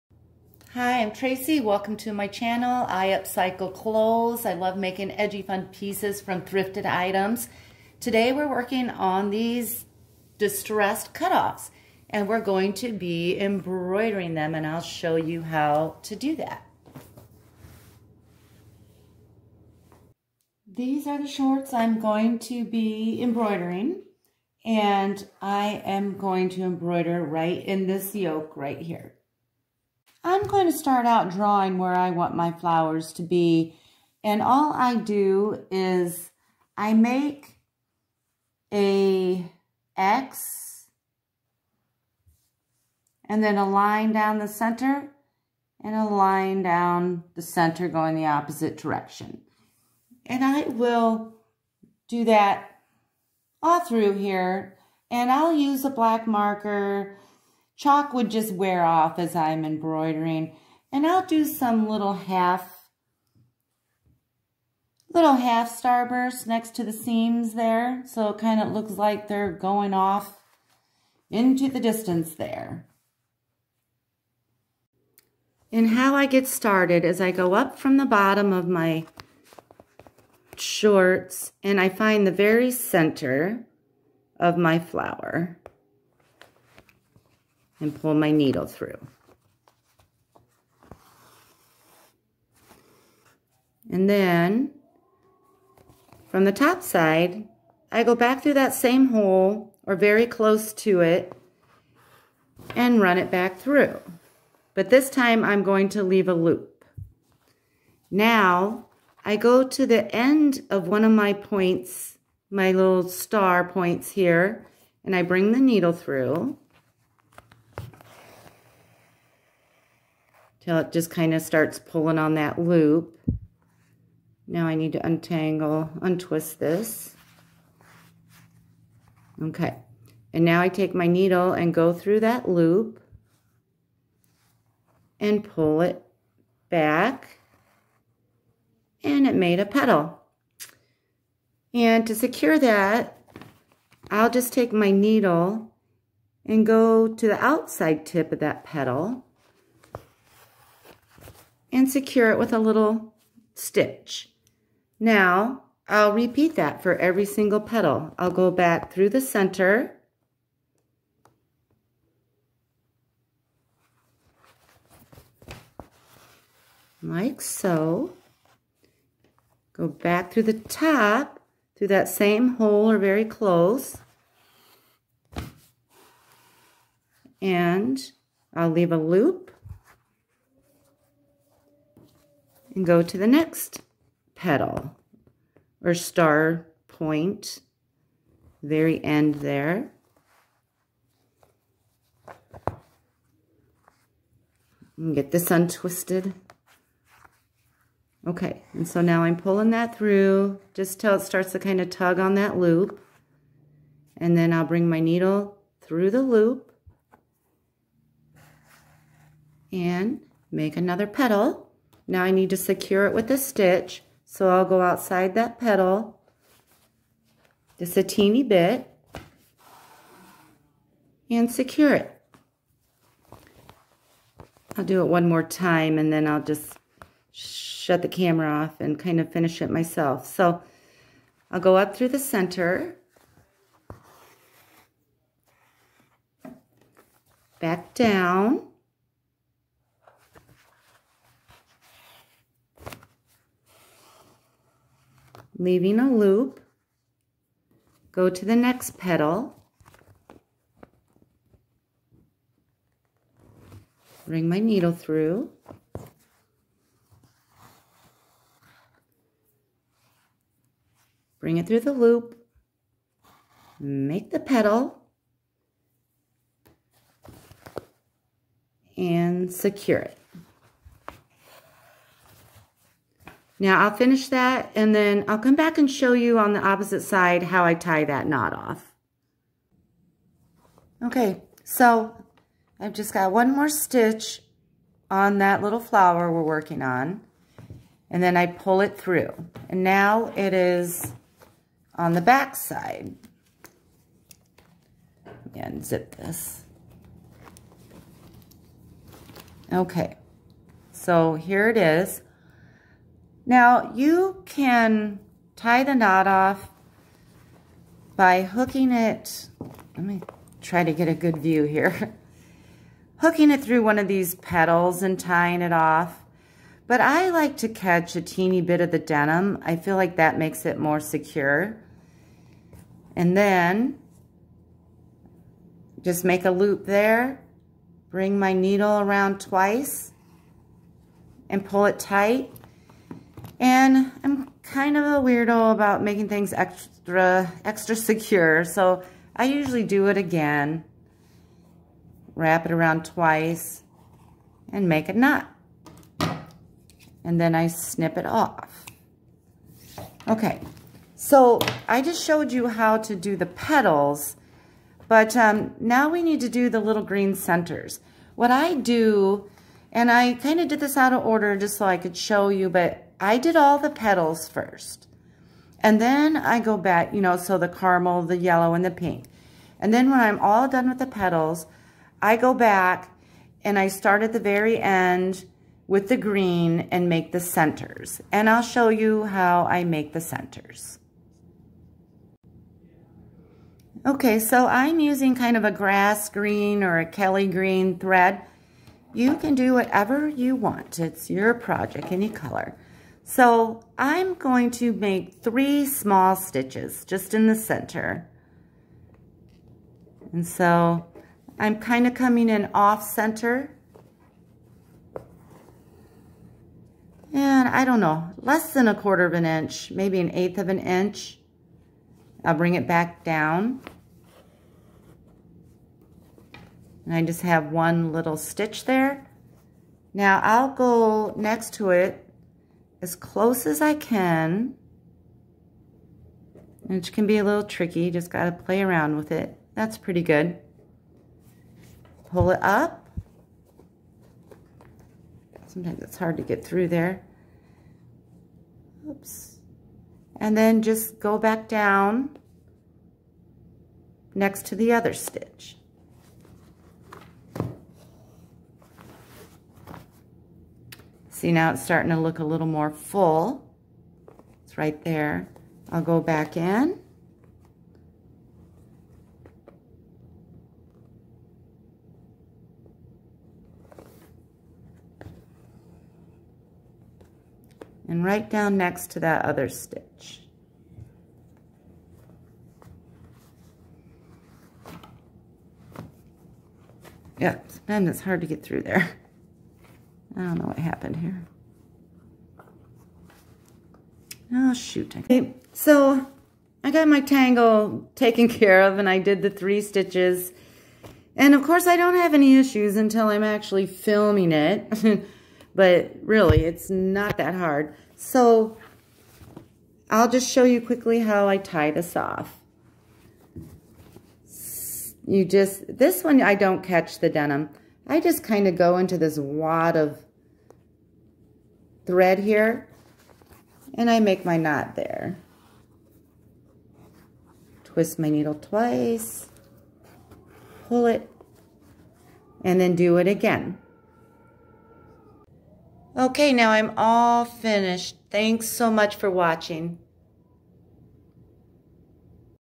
How to jazz up jean shorts with embroidered flowers.
This is a DIY tutorial on how to embroider pretty little flowers on a pair of cut off jean shorts...so cute!